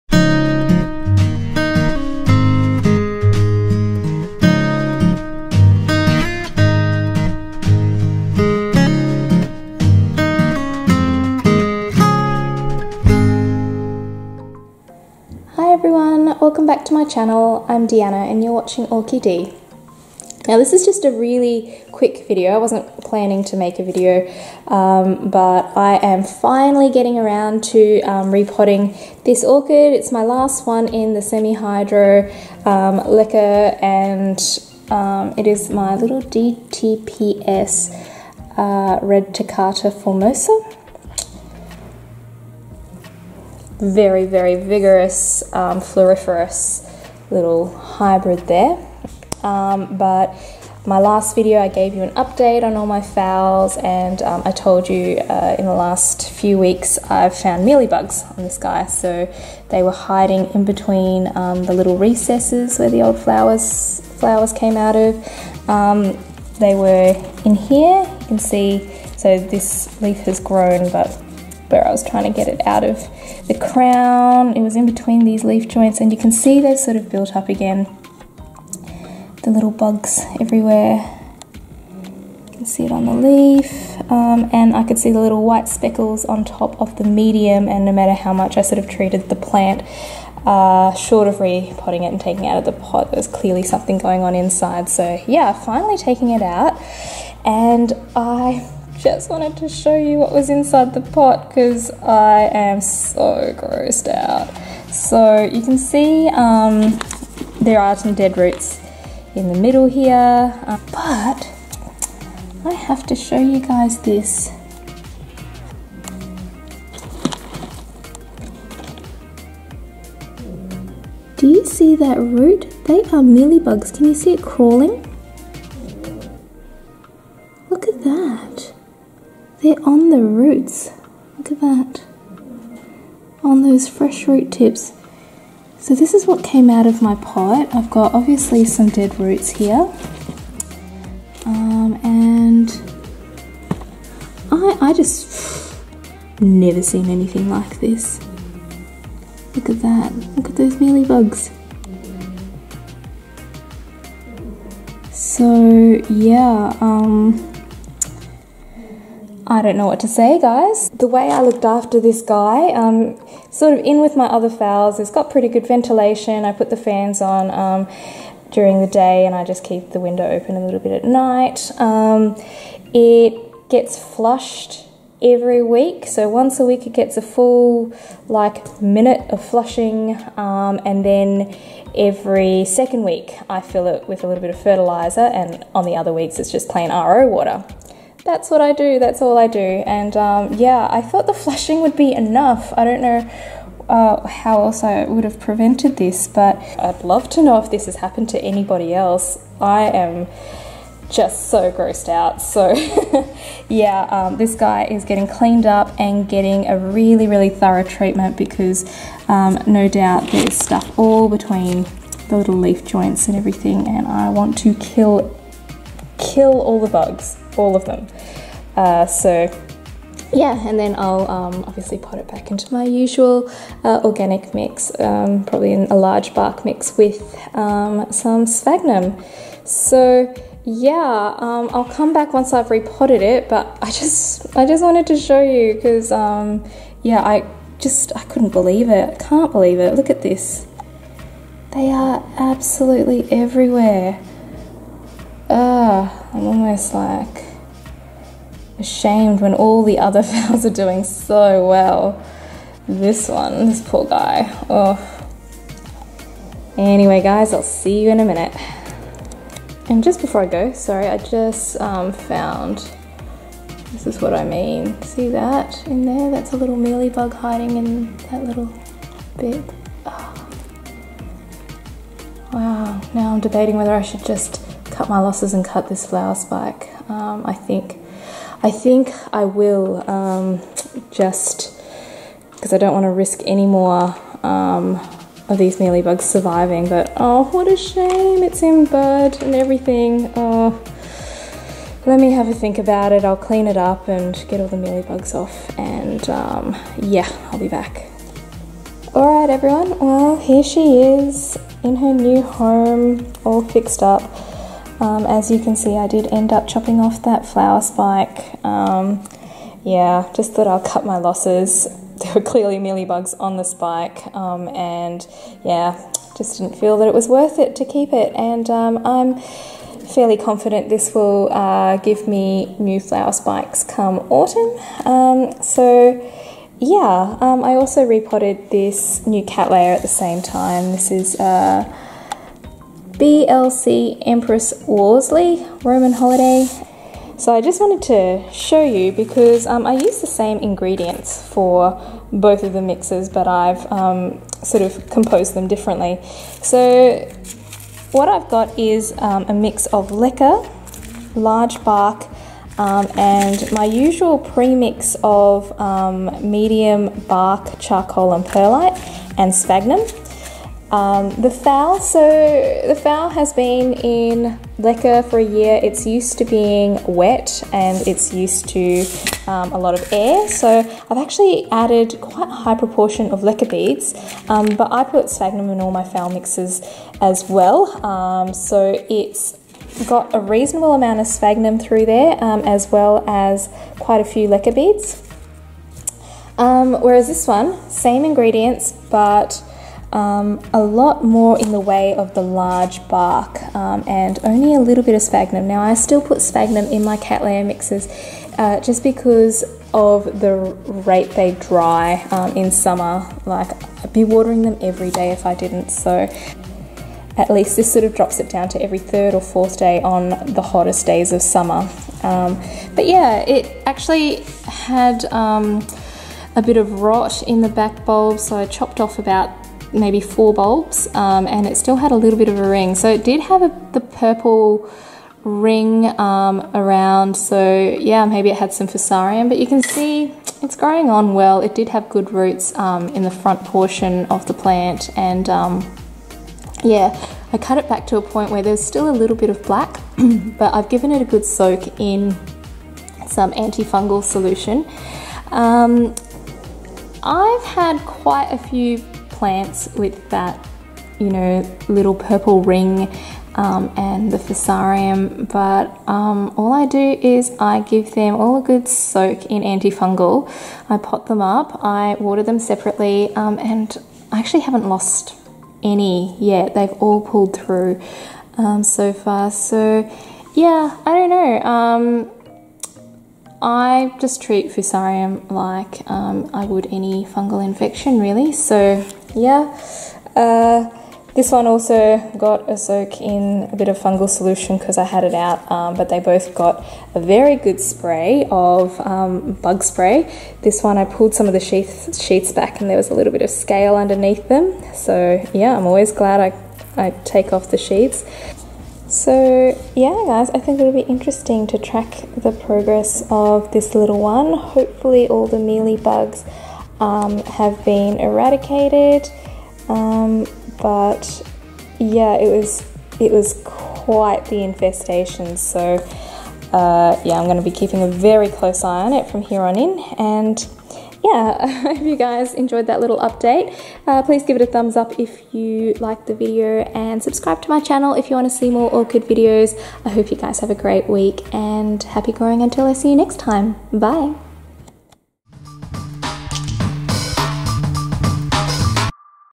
Mealy Bug Hell during Phalaenopsis Repot
The last of my Semi-Hydro setups to be converted back to organic media.  I've been delaying it for ages, but finding some mealy bugs last week has forced my hand... little did I know the nightmare that lay within the pot!!